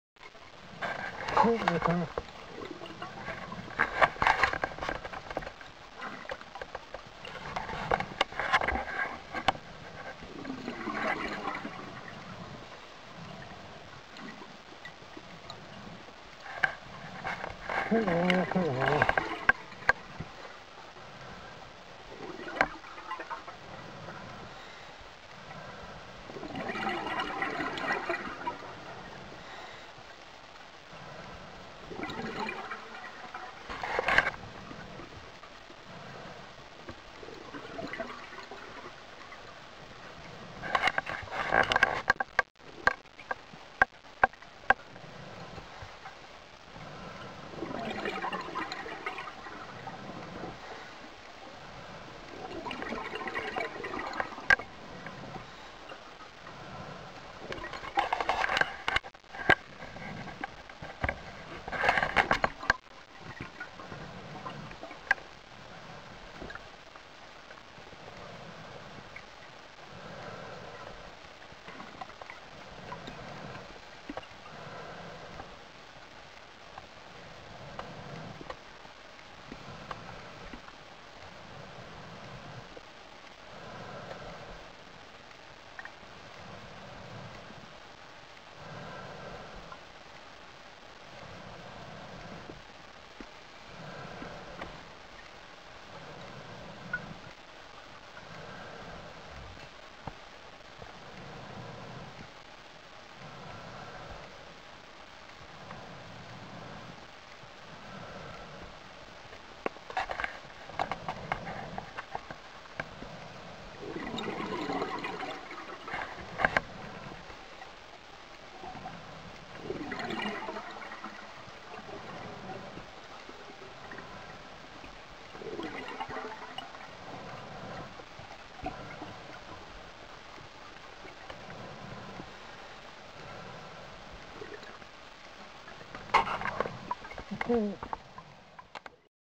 Tyee - Grandpa 02-21-2010
Video of grandpa wolf eel taken near Tyee, Parksville, Vancouver Island.